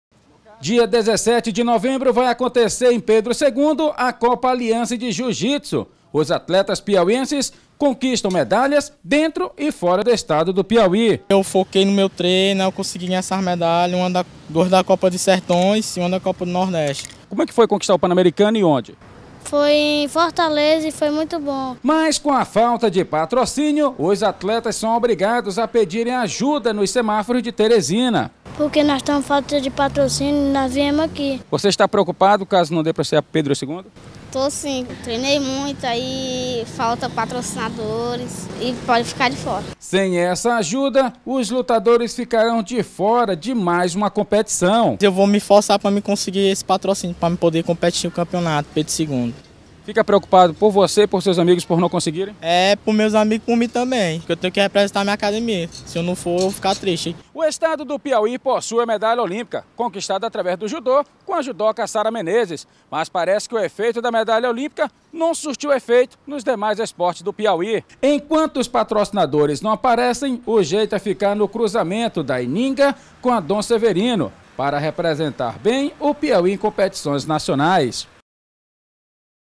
ATLETAS DO JIU JITSU VÃO ÀS RUAS EM BUSCA DE APOIO
ATLETAS DE JIU JITSU PEDEM AJUDA NOS SEMÁFOROS PARA PARTICIPAREM DE COMPETIÇÕES.